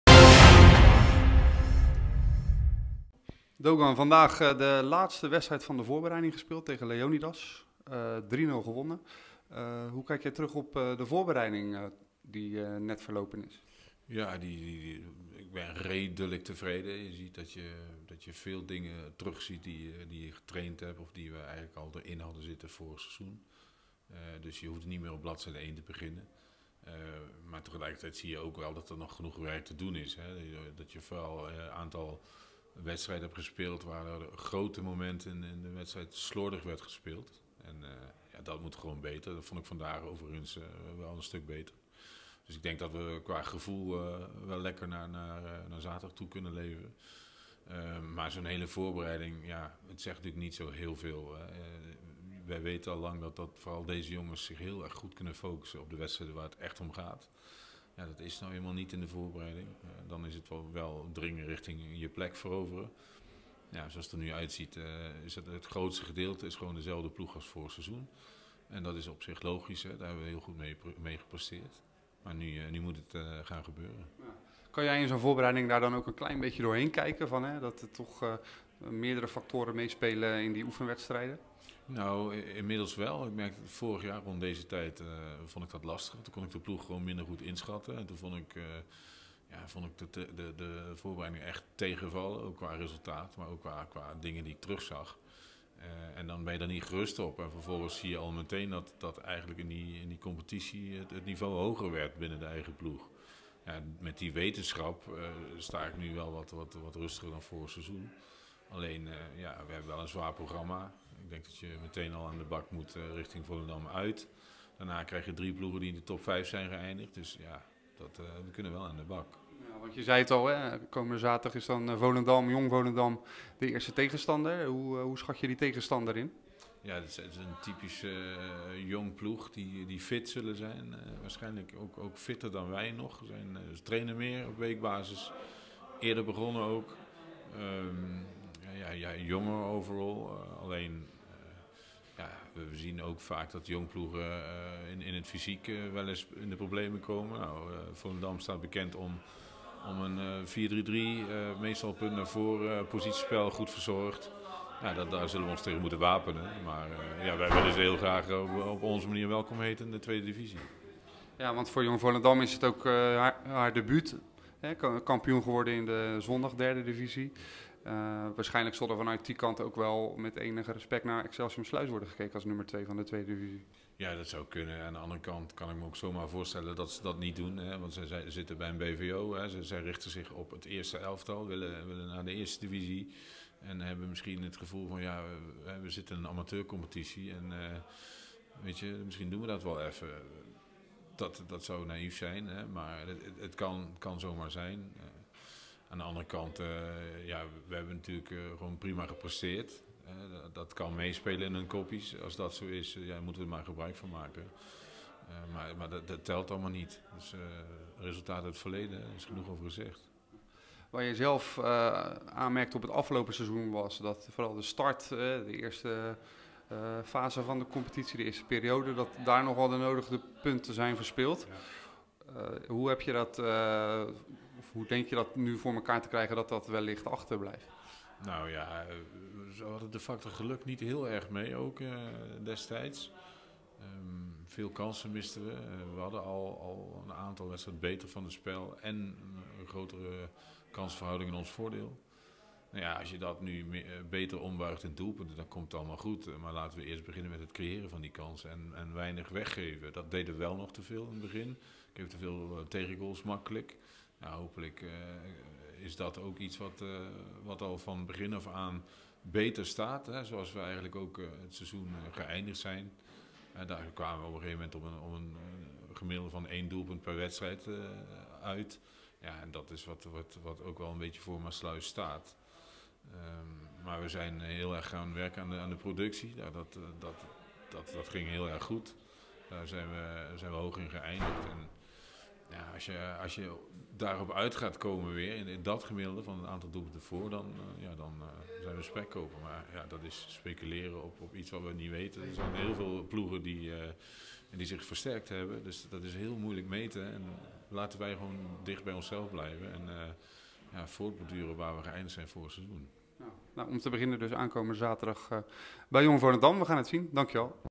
Dogan Corneille: ''We kunnen aan de bak tegen Jong FC Volendam''.
Excelsior Maassluis TV blikte met hoofdtrainer Dogan Corneille vooruit op de eerste competitiewedstrijd tegen Jong FC Volendam.